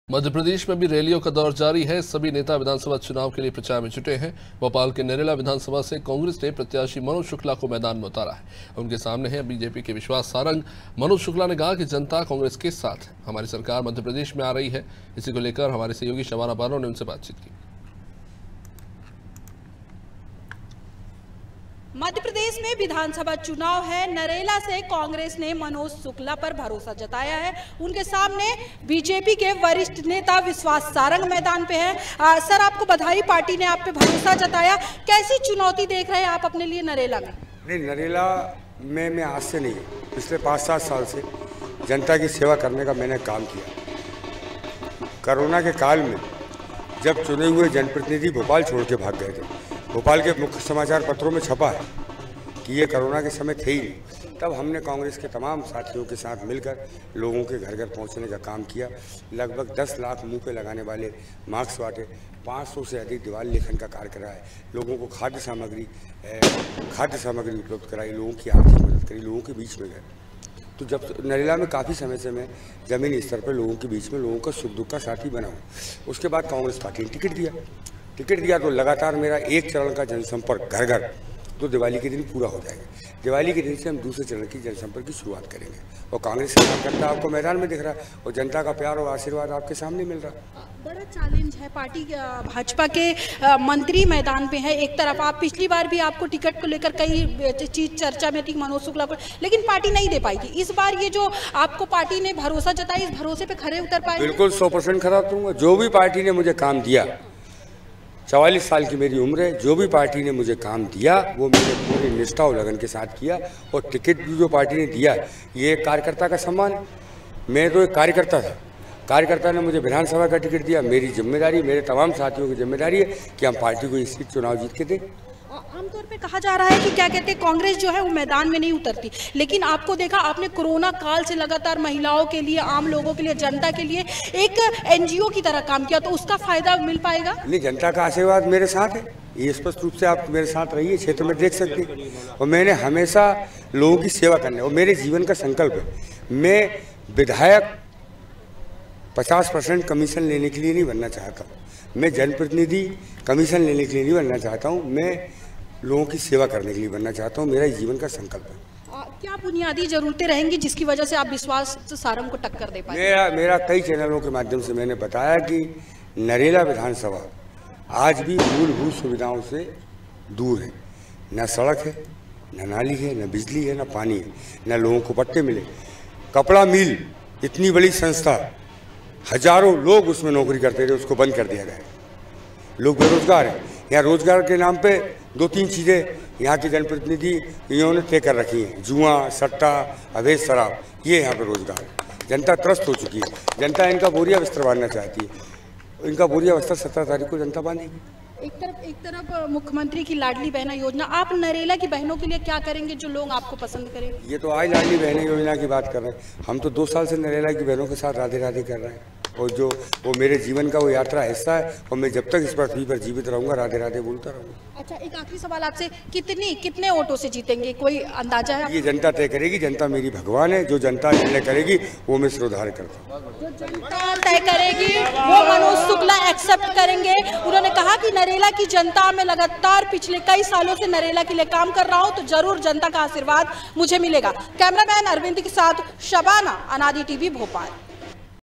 Bhopal : MP में विधानसभा चुनाव, प्रत्याशियों का चल रहा धुंआधार प्रचार || Anaadi Tv
मध्यप्रदेश में अभी रैलियों का दौर जारी हैं... सभी नेता विधानसभा चुनाव के लिए प्रचार में जुटे हैं... भोपाल के नरेला विधानसभा से कांग्रेस ने प्रत्याशी मनोज शुक्ला को मैदान में उतारा हैं... उनके समाने भाजपा के विश्वास सारंग मैदान में हैं... मनोज शुक्ला ने कहा कि, जनता कांग्रेस के साथ हैं...हमारी सरकार मध्यप्रदेश में आ रही है. इसी को लेकर अनादि tv की संवाददाता शबाना बानों ने कांग्रेस प्रत्याशी मनोज शुक्ला से जनसंपर्क के दौरान खास बातचीत की है...

COMING SOON
 DEN, AIRTEL